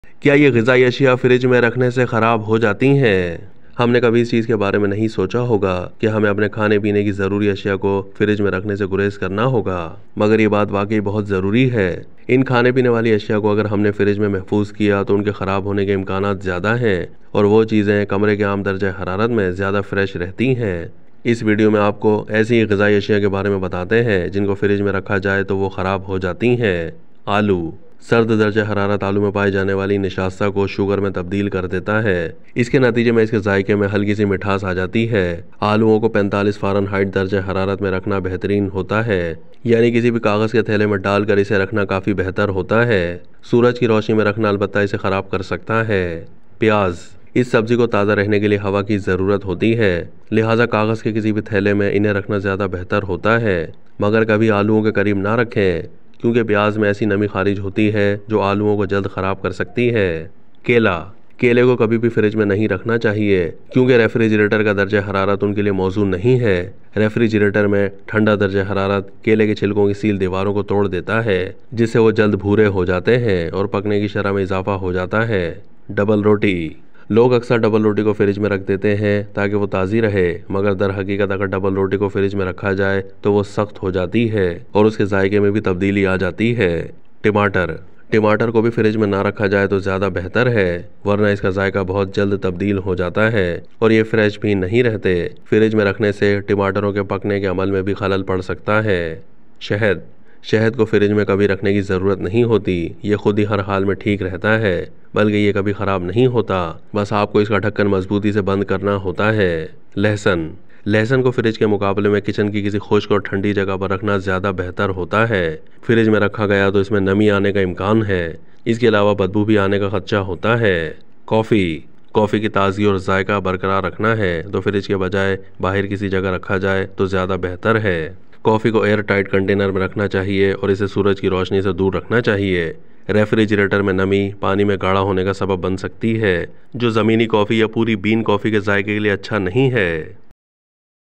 یہ غذائی اشیاء فریج میں رکھنے سے خراب ہوجاتی ہیں؟ | Foods Spoil | Fridge
Some food items are more likely to spoil if we store them in the refrigerator and they stay fresher at normal room temperature. Today, we tell you about such food items that spoil if kept in the Fridge.